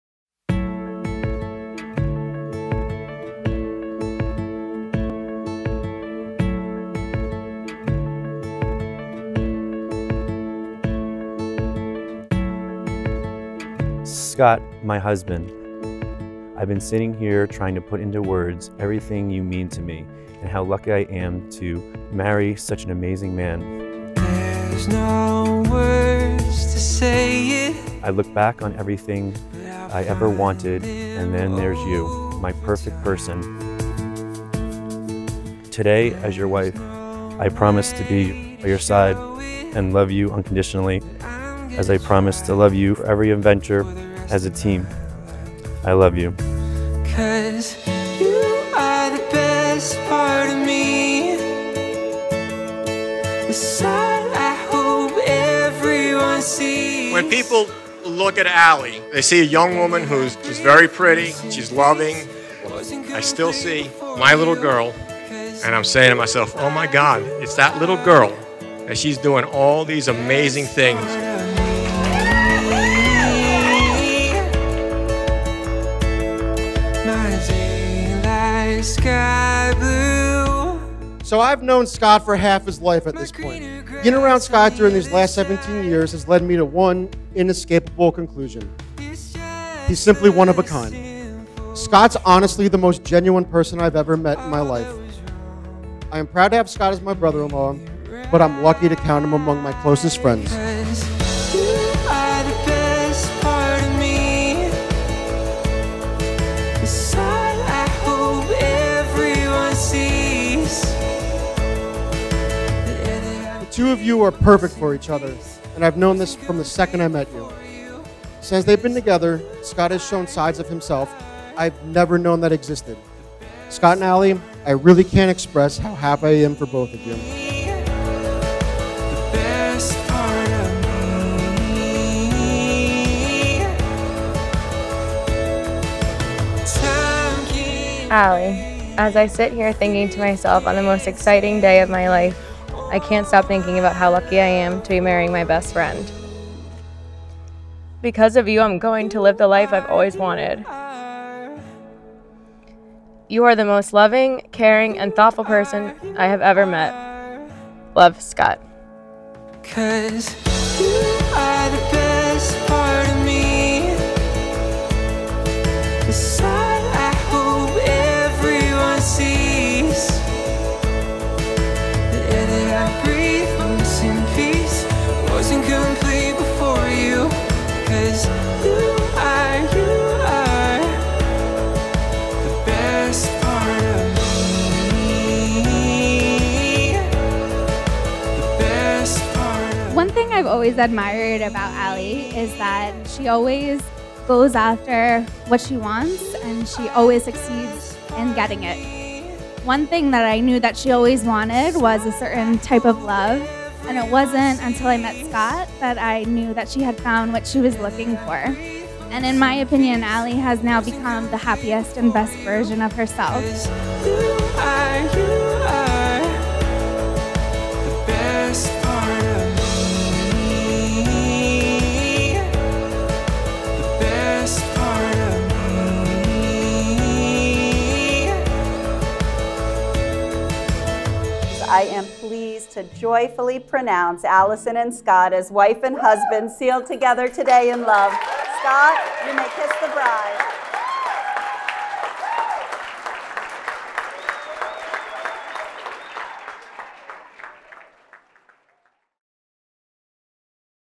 Alli and Scott Wedding Highlight at Perona Farms, NJ
Produced by Live Picture Studios
music from SongFreedom.com

Alli and Scot are the meaning of true love. So many moments, experiences and adventures together have led up to this beautiful moment. Surrounded with their closest family, friends, they celebrated into the night at Perona Farms, NJ. We were so excited to help capture their special day, Congratulations!